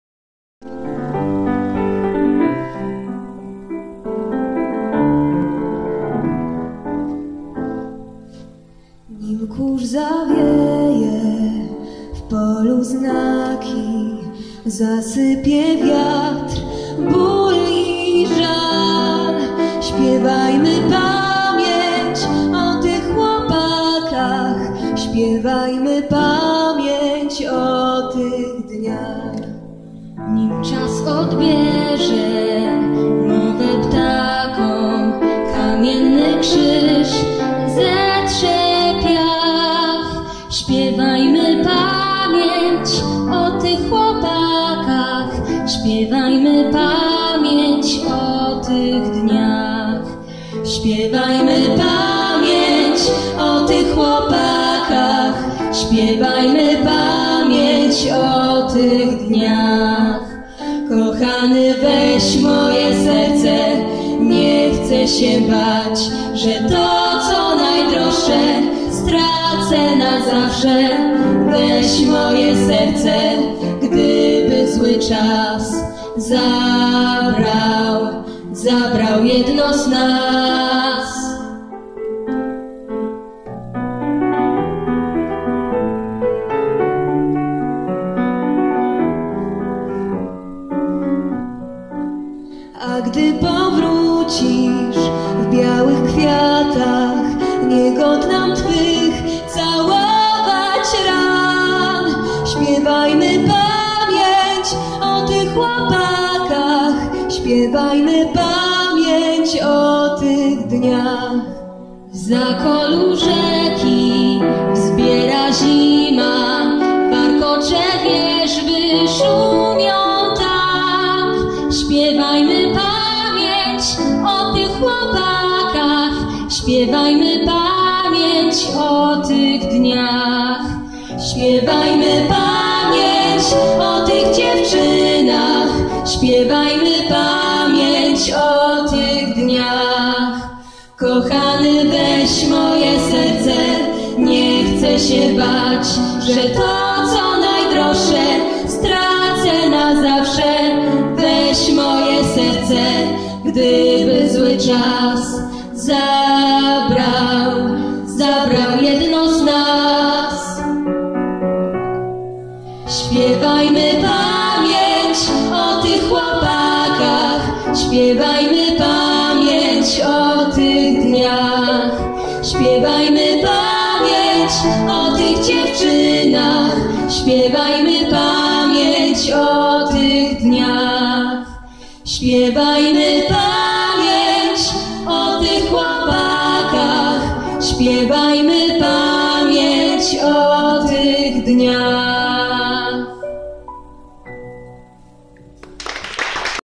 niepodlegla2018 julia zielinska ola bragiel
I miejsce w konkursie poezji i pieśni patriotycznej „Niepodległa, Niepokorna” w JDK kwiecień 2018 Ola Brągiel Julia Zielińska